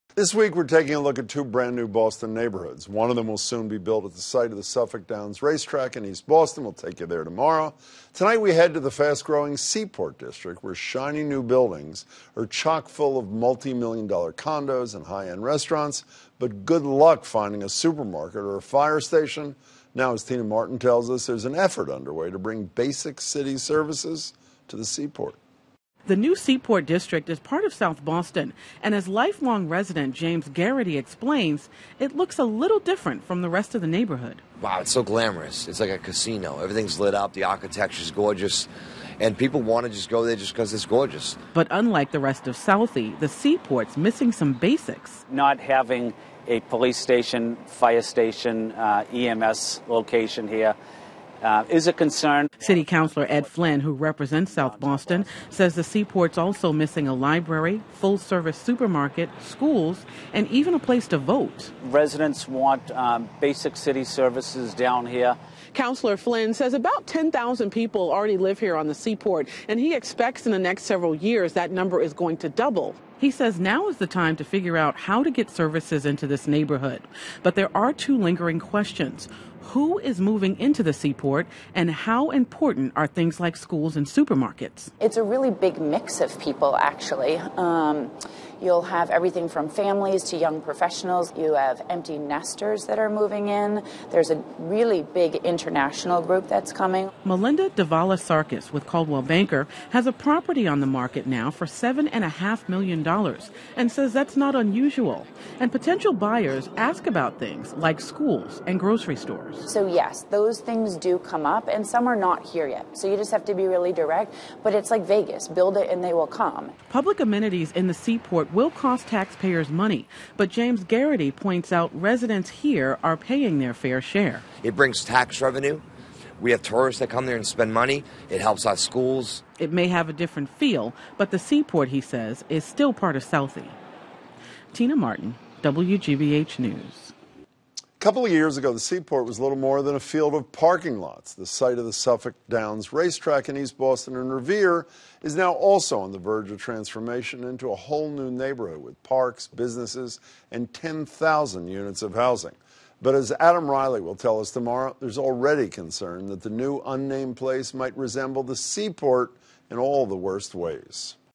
Services In The Seaport
The fast-growing Seaport district has shiny new buildings that are chock-full of multi-million-dollar condos and high end restaurants, but good luck finding a supermarket or a fire station. Now, as Tina Martin reports, there's an effort underway to bring basic city services to the Seaport.